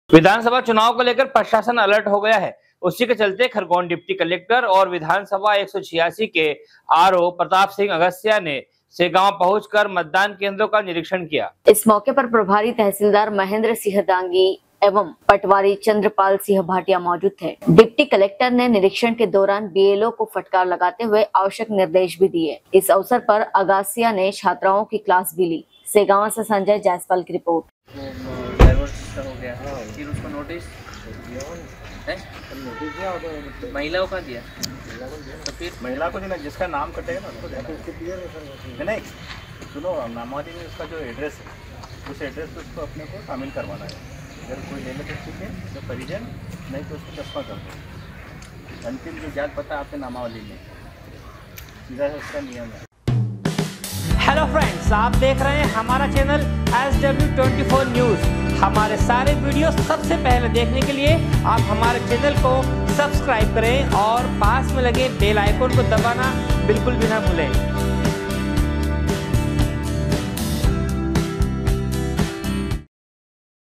डिप्टी कलेक्टर और विधानसभा 186 के आरो प्रताप सिंह अगास्सीया ने सेगांव पहुंचकर मतदान केन्द्रों
विधानसभा चुनाव को लेकर प्रशासन अलर्ट हो गया है उसी के चलते खरगोन डिप्टी कलेक्टर और विधानसभा 186 के आरो प्रताप सिंह अगास्सीया ने सेगांव पहुंचकर मतदान केन्द्रों का निरीक्षण किया।